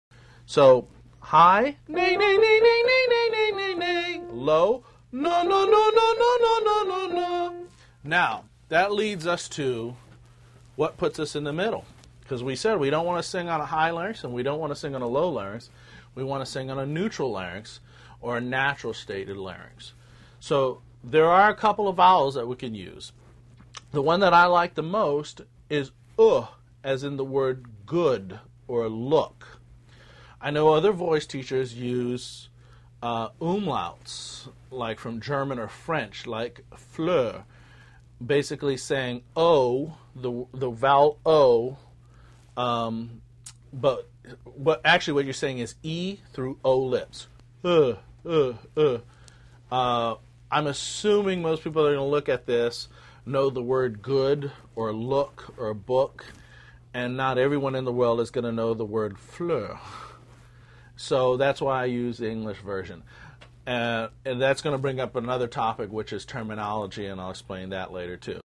Vocal Exercises With Mid Range Vowels for Larynx Conditioning
Vocal Exercises With Mid Range Vowels for Larynx Conditioning. Part of the series: Vocal Technique & Voice Training. Learn vocal exercises with mid range vowels for larynx conditioning and vocal exercise with singing and vocal training tips in this free online vocal coaching video clip.